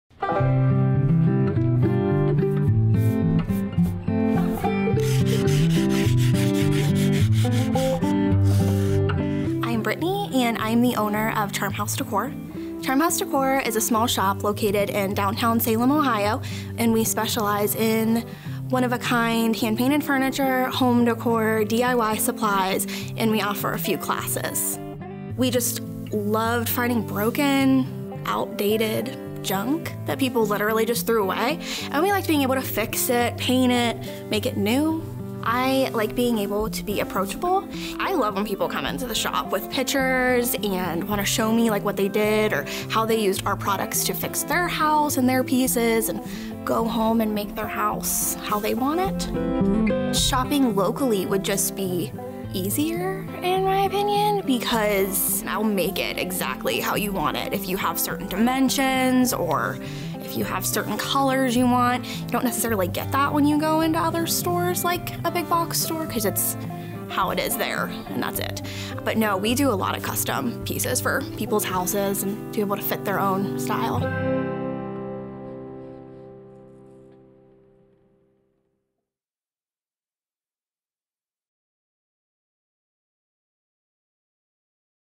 Charm House Decor | Shop Local Mahoning Valley
Charm House Decor is a shop in Salem specializing in hand painted furniture, home decor, DIY supplies and classes. They love taking discarded pieces that others don't want and breathing new life into them.

Produced by Pebble

@pebbledotmedia on Facebook, Instagram and Twitter

Shot and edited by Bill Rusu
Camera - Panasonic GH4
Sound - Rode NTG-2